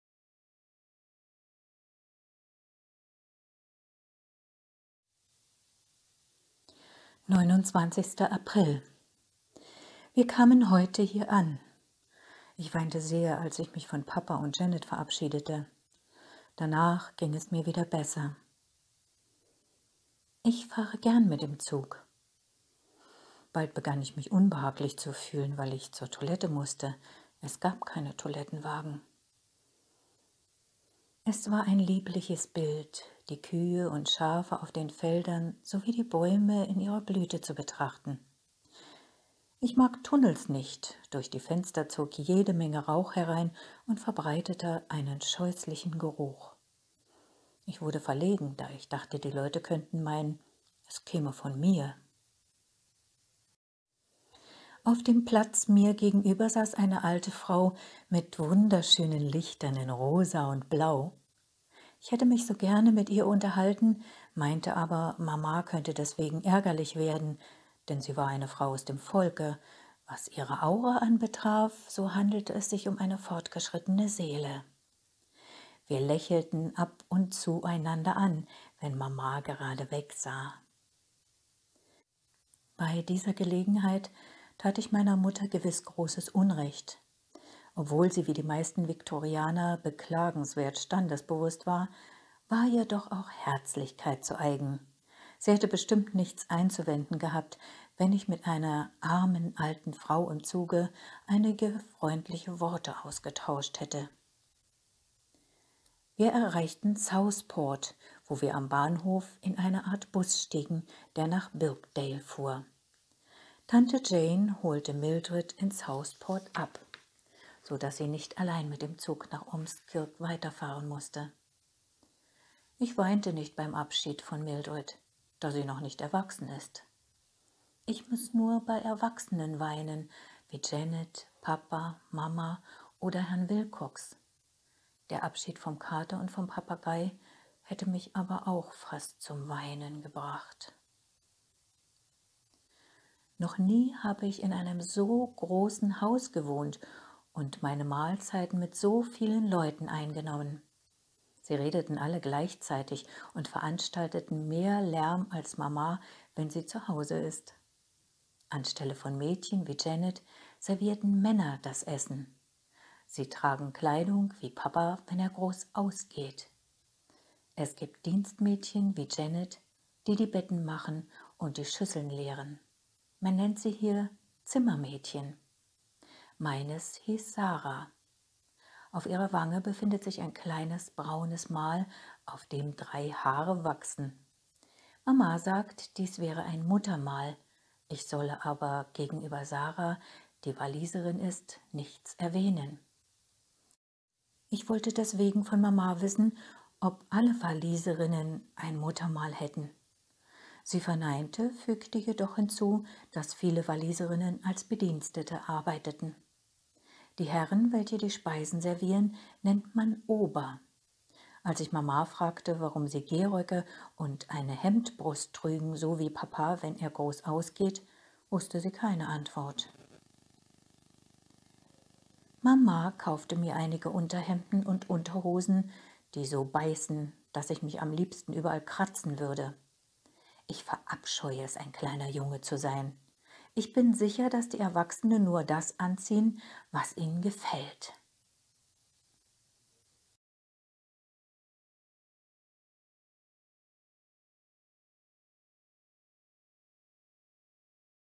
28 - Der hellsichtige Junge schreibt am 29. April 1885
Der Abschied von Erwachsenen bringt mich zum Weinen. Im Zug saß auf dem Platz mir gegenüber eine alte Frau mit schönen Lichtern in Rosa und Blau.